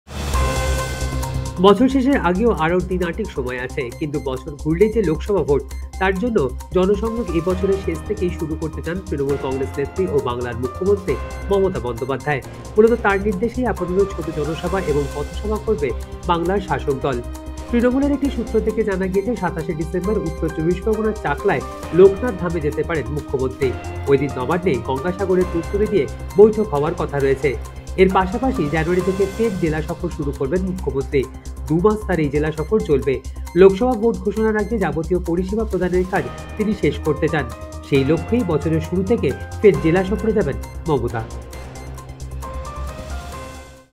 Mamata Banerjee : বছর শেষের আগেই লোকসভার প্রচার, জানুয়ারি-ফেব্রুয়ারিতে ফের জেলা সফরে মমতা
তৃণমূলের একটি সূত্র থেকে জানা গিয়েছে, ২৭ ডিসেম্বর উত্তর ২৪ পরগনার চাকলায় লোকনাথ ধামে যেতে পারেন মুখ্যমন্ত্রী। ওই দিন নবান্নে গঙ্গাসাগরের প্রস্তুতি নিয়ে বৈঠক হওয়ার কথা রয়েছে।